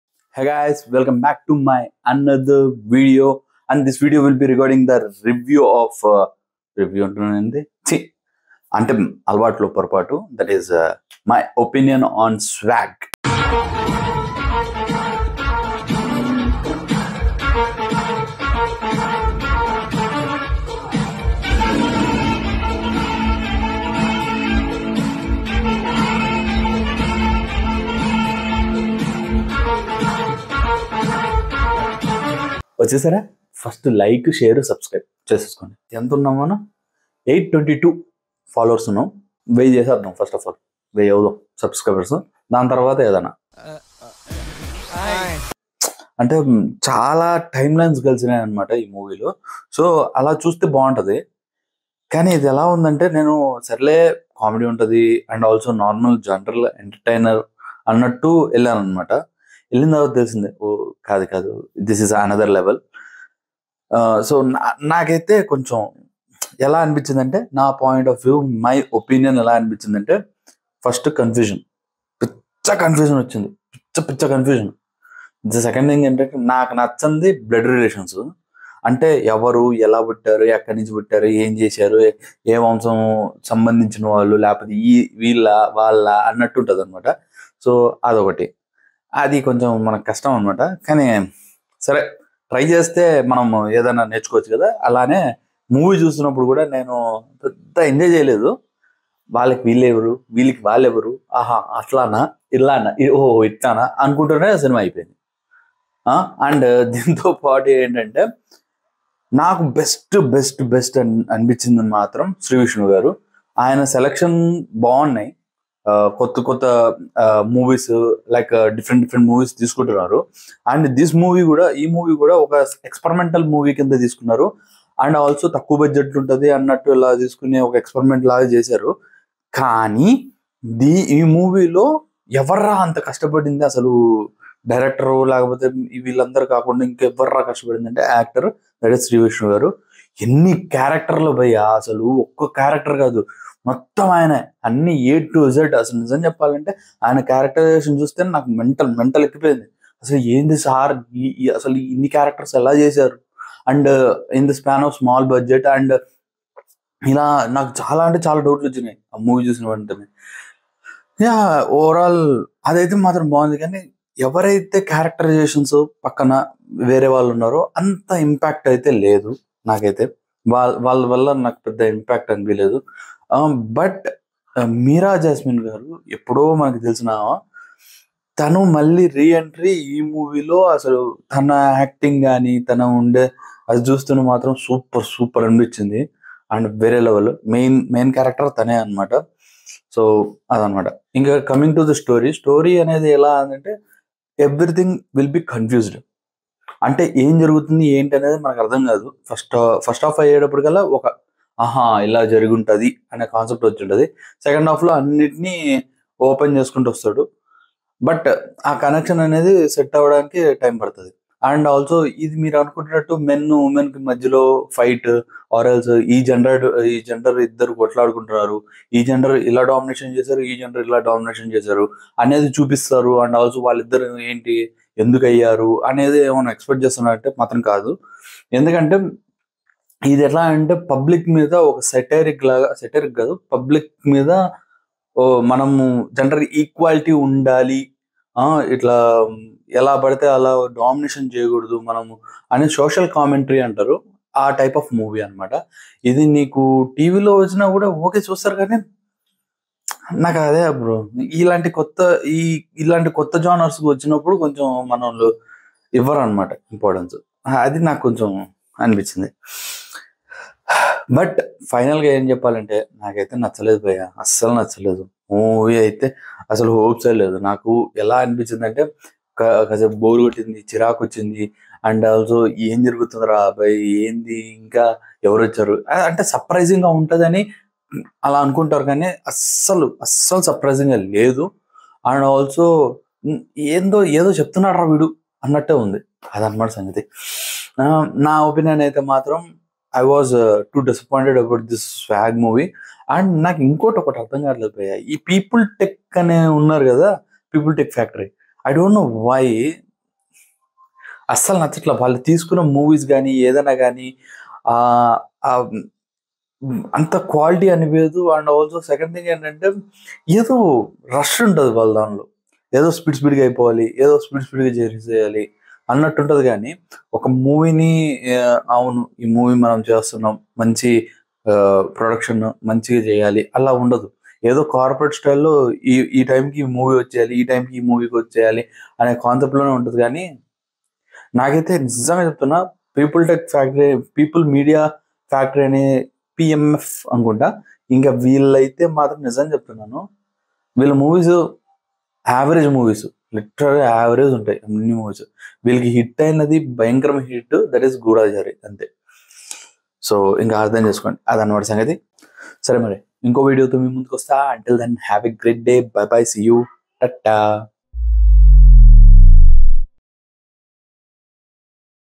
My Opinion on SWAG Movie | Sreevishnu, Meerajasmine, Rituvarma
Here comes my opinion on swag movie starring sreevishnu meerajasmine rituvarma directed by hasithgoli

You can message me here